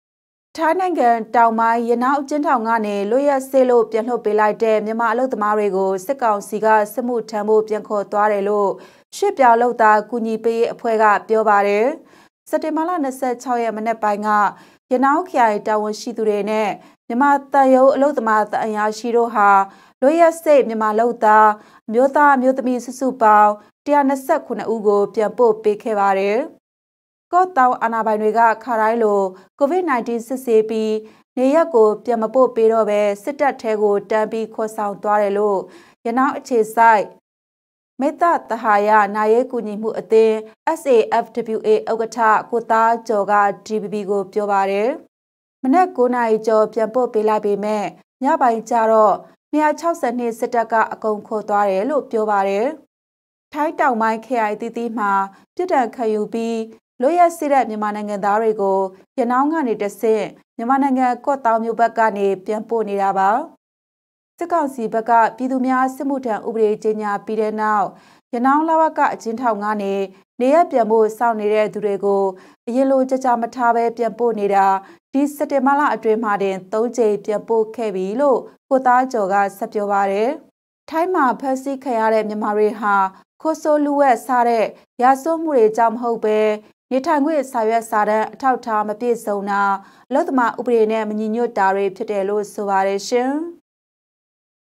ထိုင်းအကျဉ်းထောင်က ပြန်လွှတ်ပေးလိုက်တဲ့မြန်မာတွေ စစ်မှုထမ်းဖို့ခေါ်ဆောင်ခံရ- DVB News
ထိုင်းတောင်ပိုင်း၊ ရနောင်းအကျဉ်းထောင်ကနေ လွတ်ရက်စေ့လို့ ပြန်လွှတ်ပေးလိုက်တဲ့ မြန်မာအလုပ်သမားတွေကို စစ်ကောင်စီက စစ်မှုထမ်းဖို့ ပြန်ခေါ်သွားတယ်လို့ ရွှေ့ပြောင်း လုပ်သားကူညီပေးရေးအဖွဲ့က ပြောပါတယ်။